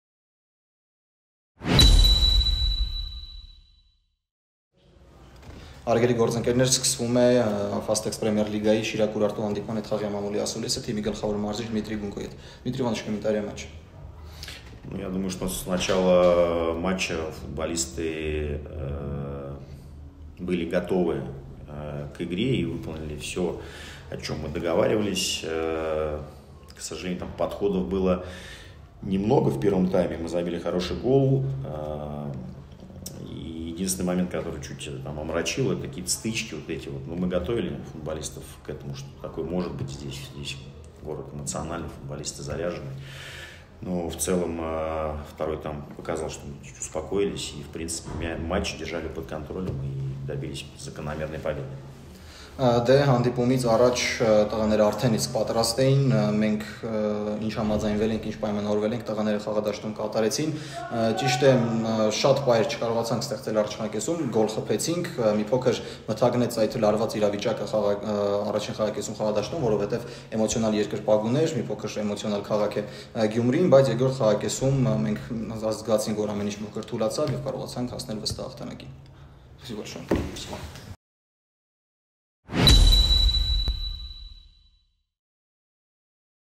Fastex PL, Matchday 6 | SC Shirak - Urartu FC 0-2 | PRESS CONFRENECE
After the sixth match of Fastex Premier League season 24/25, against SC Shirak (0-2) the head coach of Urartu FC Dmitry Gunko shared his thoughts about the match.